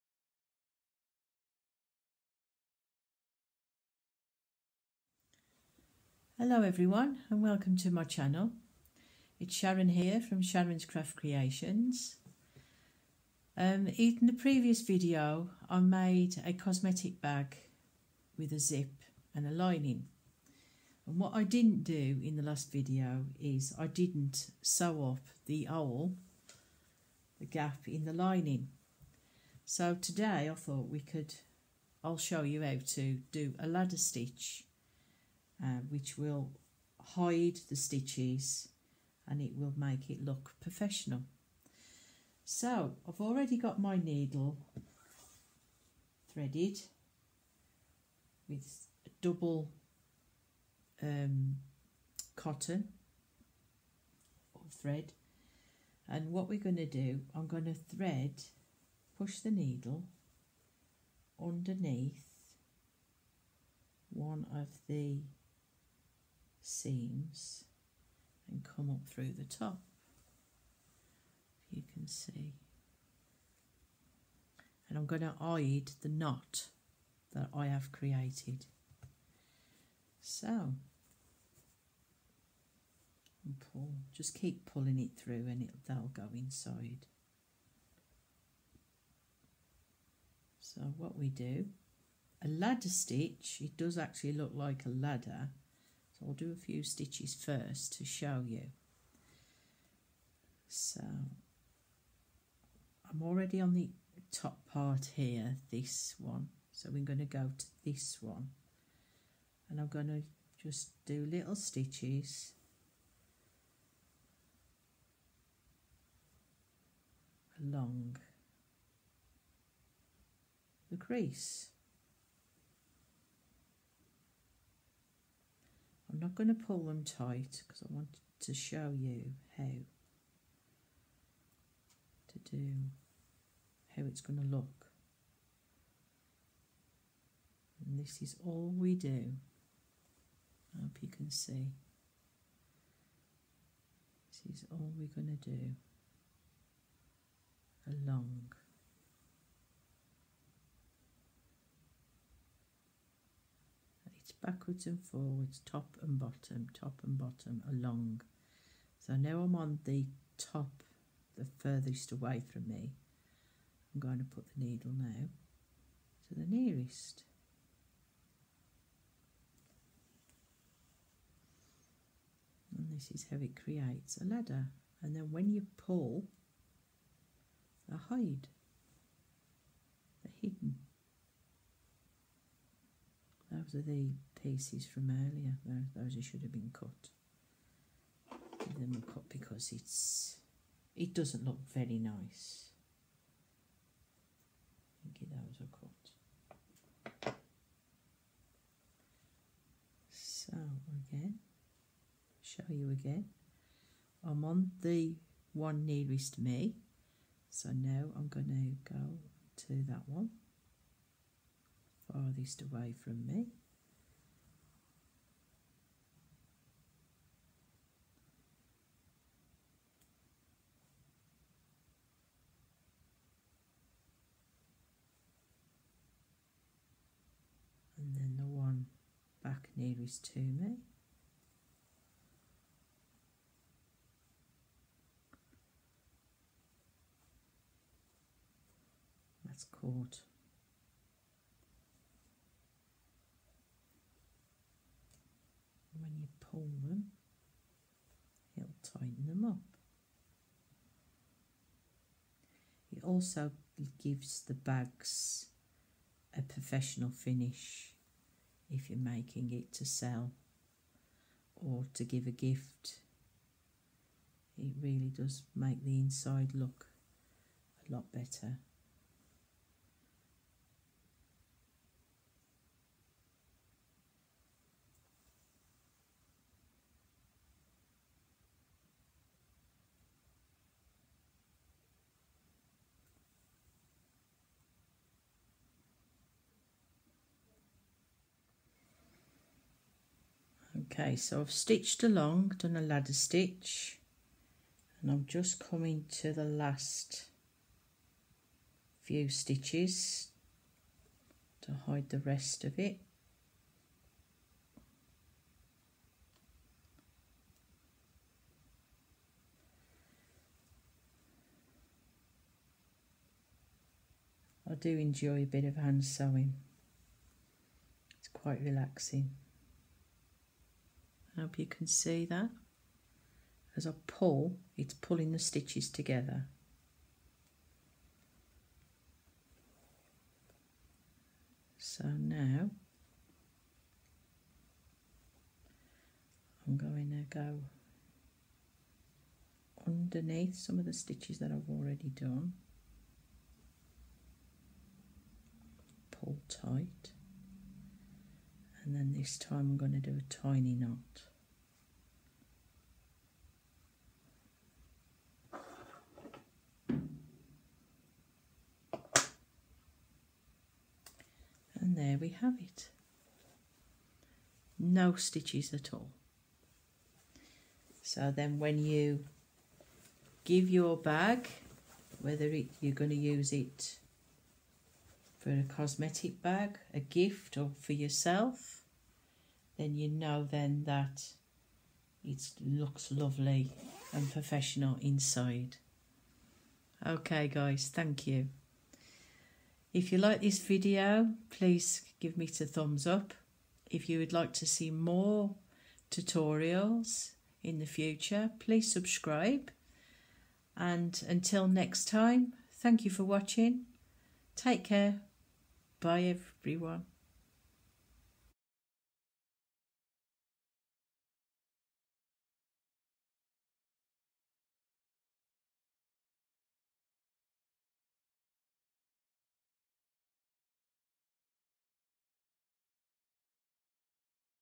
How to sew the gap in a cosmetic bag lining.
In this video i sew up the gap in a lining from another video.